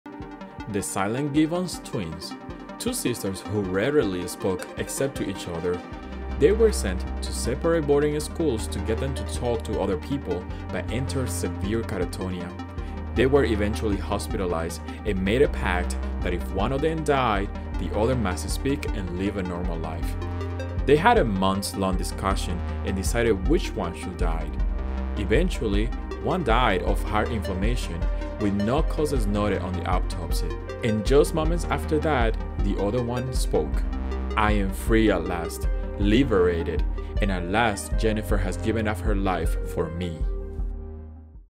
SCARIEST True Story: The Gibbon Twins
The Gibbon Twins' story is so scary because it is based on a true story. Some of these stories be having me wonder how true they actually are, but this one I Googled it, and it is true!